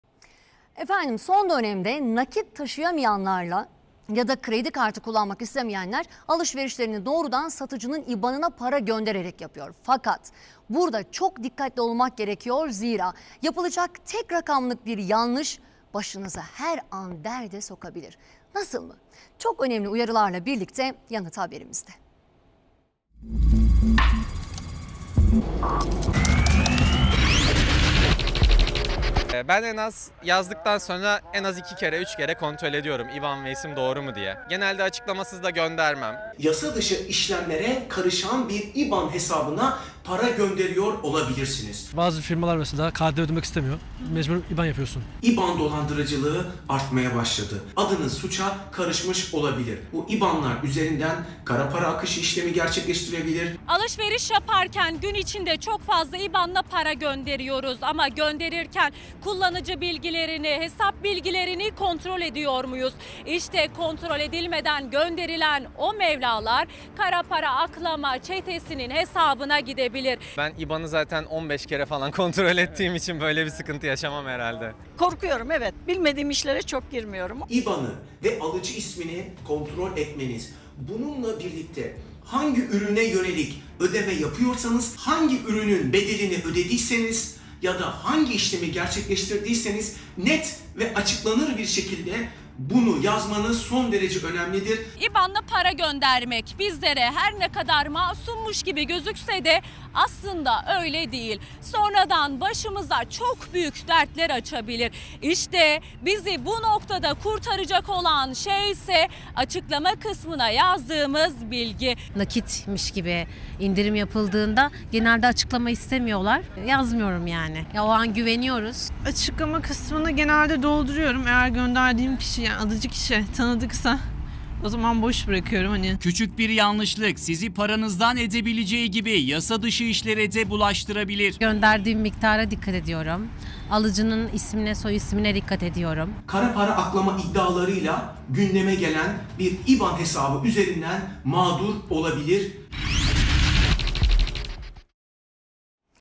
BEYAZ TV | Doç. Dr. Ali Murat Kırık | IBAN ile para gönderirken dikkat! Adınız suça karışabilir!
Son dönemlerde  kredi kartı kullanmak istemediğimiz durumlarda IBAN numarası ile ödeme yapabiliyoruz. Fakat bilmediğiniz bir IBAN'a para göndererek  kendinizi bir anda 'kara para' trafiğinin ortasında bulabilirsiniz.

Dr. Ali Murat Kırık konuya yönelik görüşlerini BEYAZ TV'den Büşra Nur Danişment'e aktardı, Hakan Kılıçarslan ise görüntüledi. (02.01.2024)